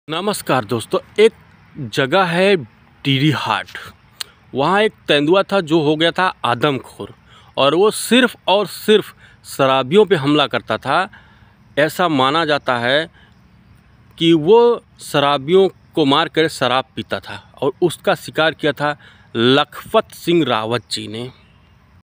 Alcoholic monitor#nature wild life#jangal#leopard#Lakhpat Singh rawat#yuvtube shorts
डीडी हाट का शराबी तेंदुआ।जो की लखपत सिंह रावत जी ने उसका शिकार किया था।
d d hot moniter leopard
lakhpat Singh rawat
adm khor leopard
alcoholic moniter
wild animals